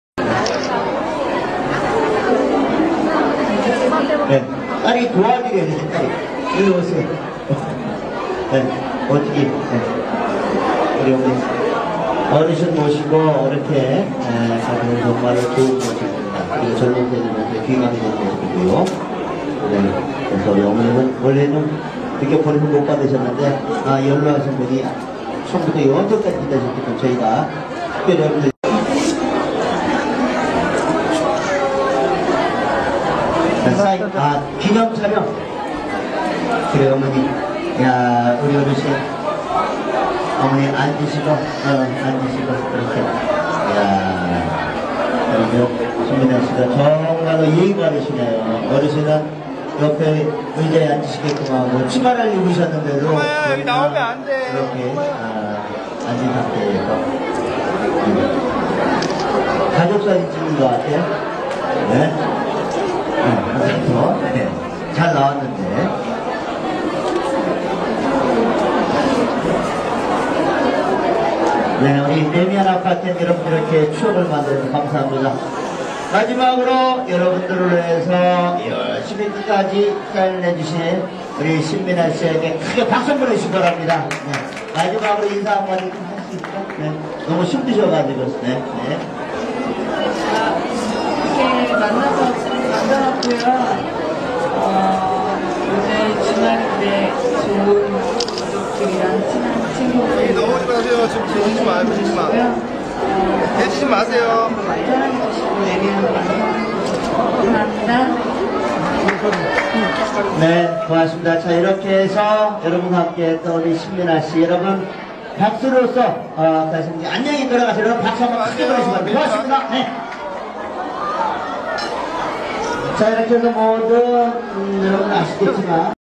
[Shin Min-A] 20120310_Raemian Fan Signing Event(2/2)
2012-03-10. Autograph Sessión for 'Raemian' in Model House
(Gimpo-si, Gyeonggi-do)

[친절한 자막 제공~ : ) ]

"이렇게 만나서 반가웠고요,
오늘 주말인데 가족분들이랑 친한 친구분들이랑
좋은 시간 보내시고요,
앞으로도 많이 사랑해 주시고,
래미안도 많이 사랑해 주세요~
감사합니다."